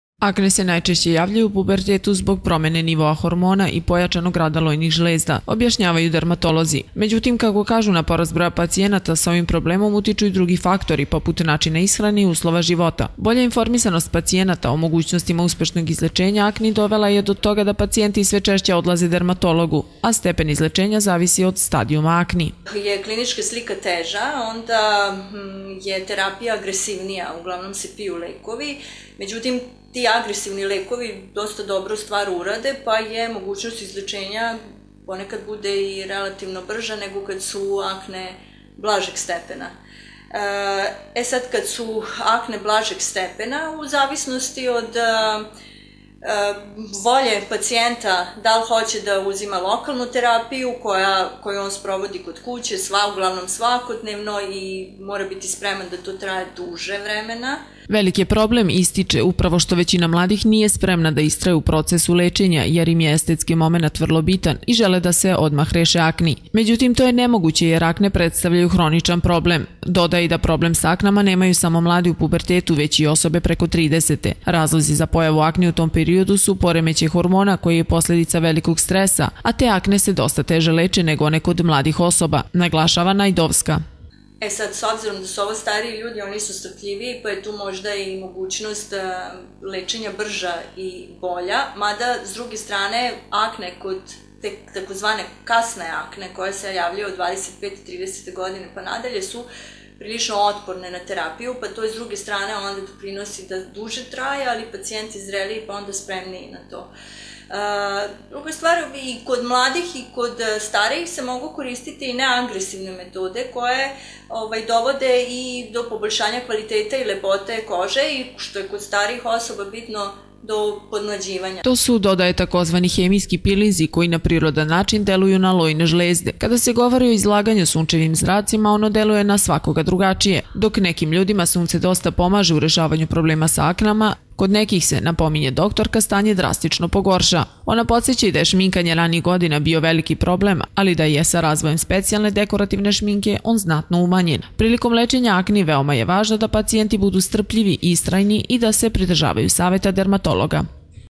TV KANAL 9, NOVI SAD: ZA LEČENJE AKNI POTREBNO STRPLJENJE
Sve više mladih ljudi javljaju se dermatolozima zbog problema sa aknama. Pojava akni u pubertetu je normalna, ali je važno da se mladi pridržavaju preporučenih terapija kako bi se problem rešio, kažu stručnjaci. Mnogo je, međutim, veći problem kada se akne pojave kod starijih osoba, jer je to znak da je došlo do nekog poremećaja u organizmu.